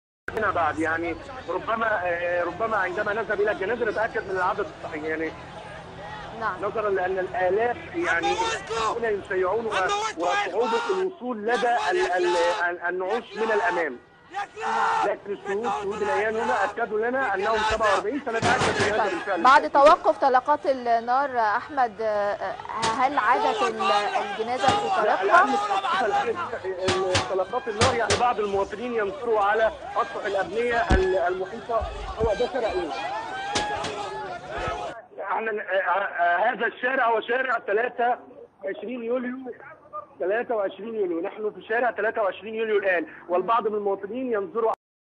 تحطيم كاميرا الجزيرة أثناء تغطية جنازة ضحايا بورسعيد
تحطيم كاميرا الجزيرة أثناء تغطية جنازة بورسعيد علي الهواء